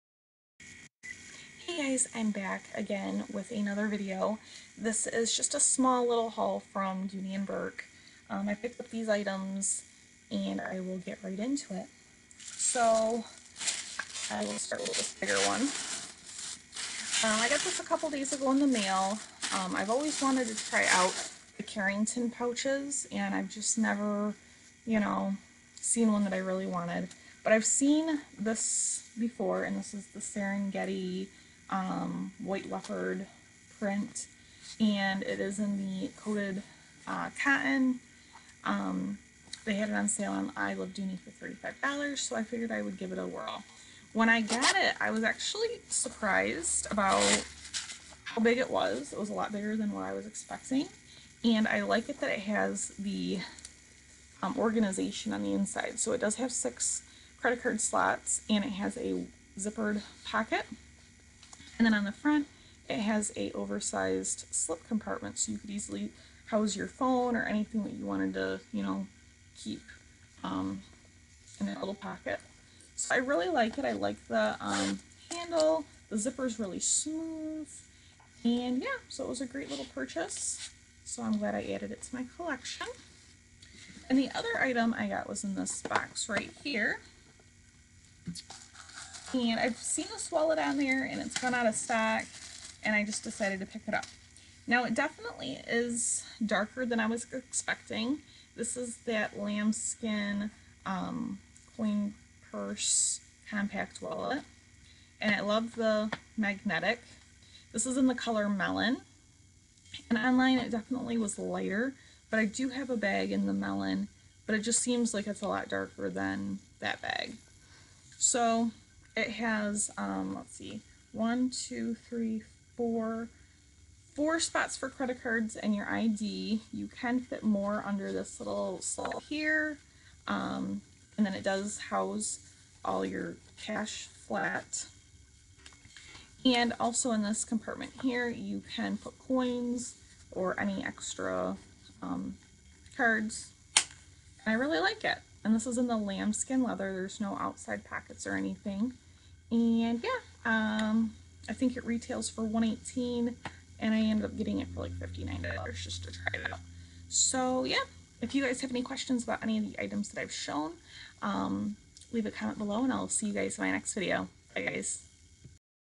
Small Dooney and Bourke Haul!!
Here is a quick little Dooney haul. These items were purchased on I Love Dooney.